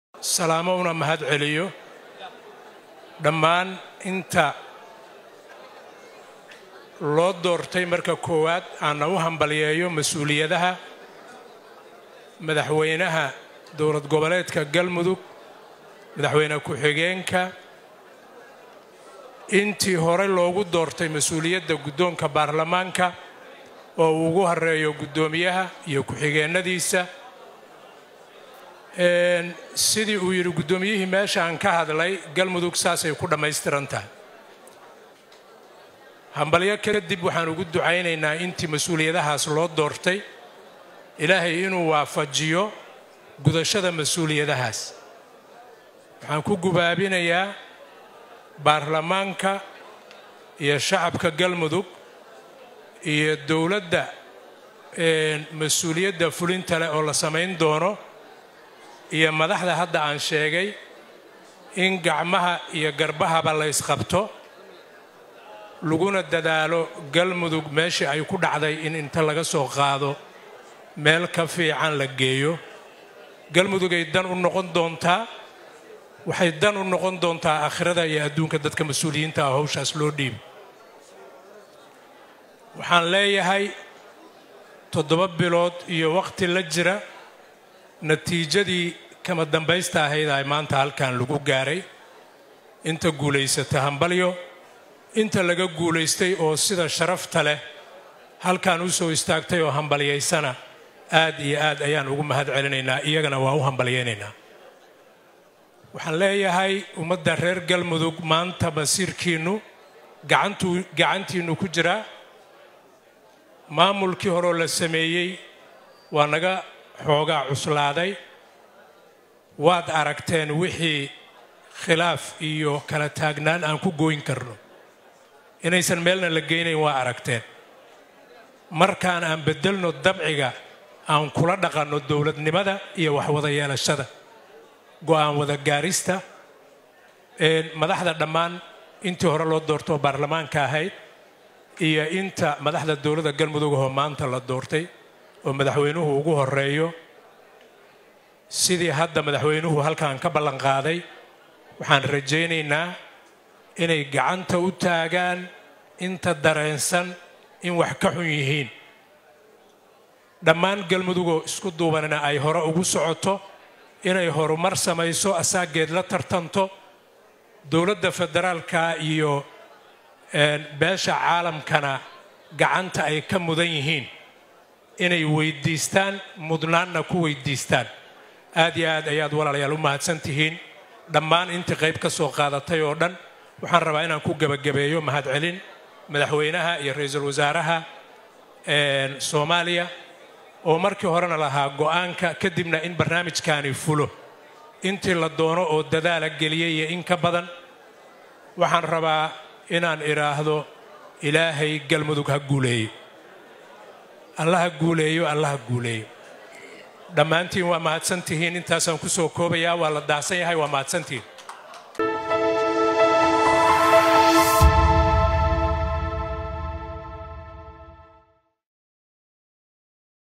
WASIIRKA ARIMAHA GUDAHA MUXUU KA YIRI DOORASHADII GALMUDUG?
Wasiirka arimaha gudaha Soomaaliya Cabdi Maxamed Sabriye ayaa khudbad ka jeediyay markii ay dhacday doorashadii madaxtinimo ee Galmudug.